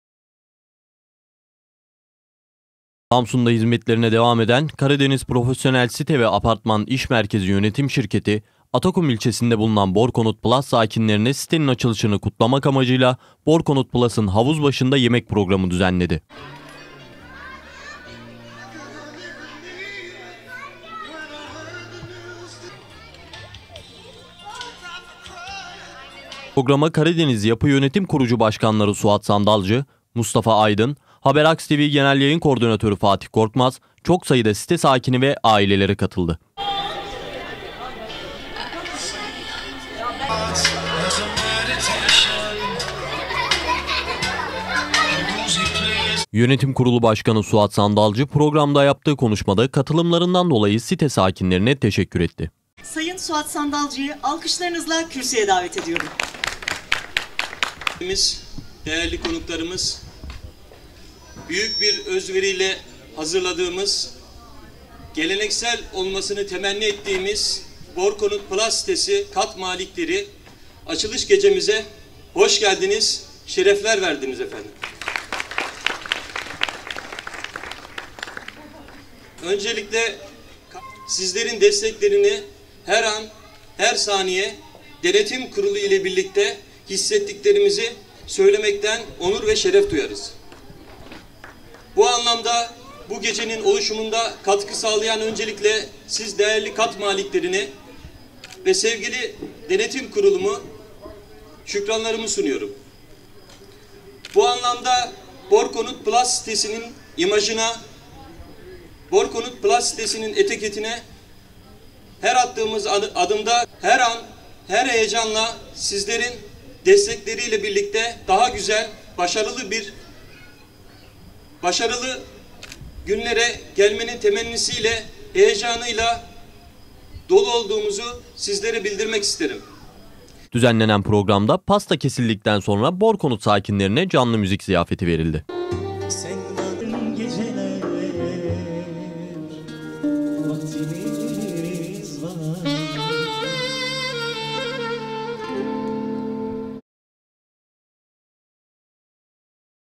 KARADENİZ YAPI'DAN BORKONUT PLUS'A GÖRKEMLİ GECE
Samsun’da hizmet veren Karadeniz Profesyonel Site ve apartman & iş merkezi yönetimi düzenlenen yemek organizasyonuyla Borkonut Plus sitesi sakinleri ile biraraya gelerek sitenin açılışını kutladı.